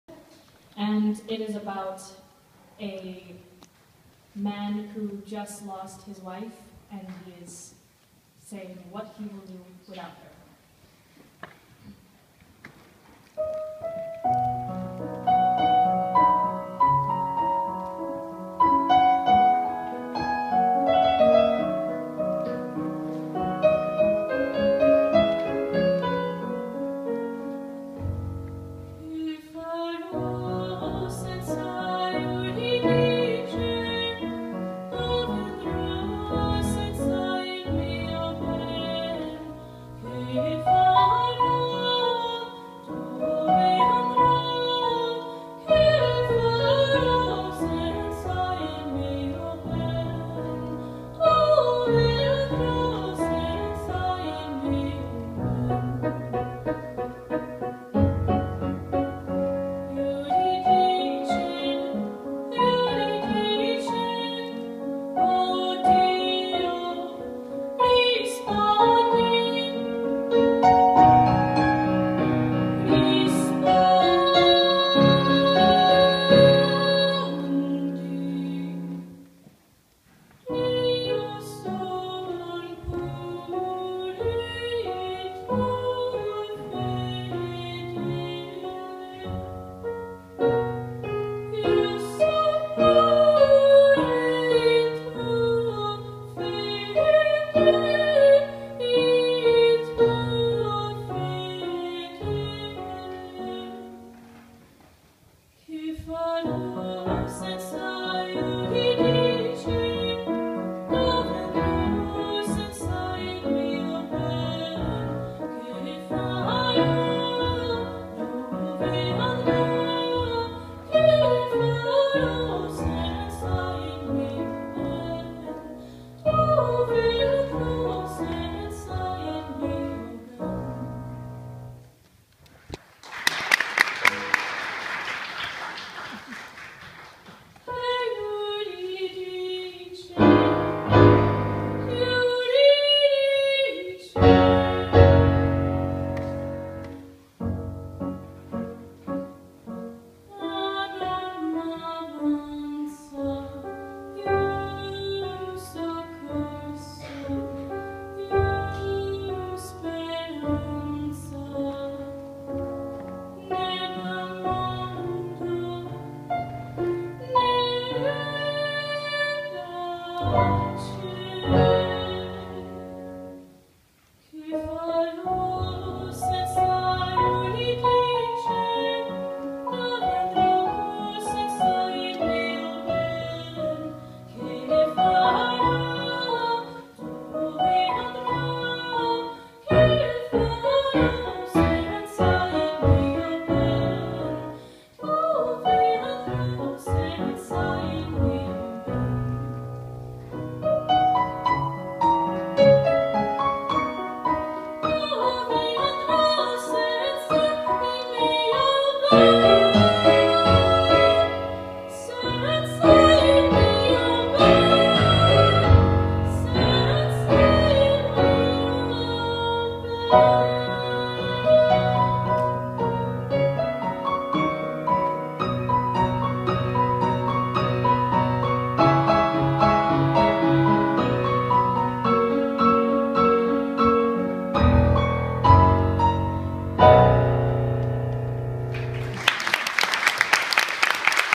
Audrey Sings Opera #2
Performed at the American International School Zagreb, Croatia, as part of her IB Music recital.